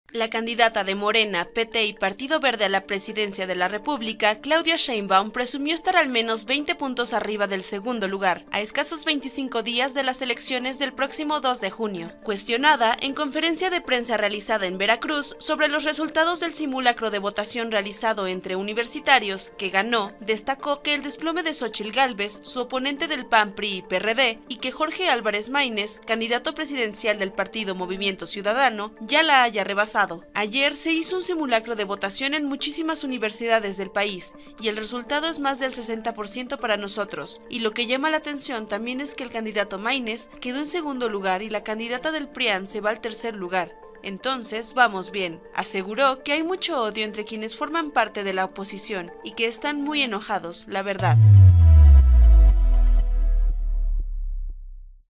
Sheinbaum presume ir a la cabeza de la encuestas rumbo a las elecciones del 2 de junio
En conferencia de prensa desde Veracruz, la candidata del oficialismo aseguró que Xóchitl Gálvez ya se desplomó al tercer lugar, superada por Máynez.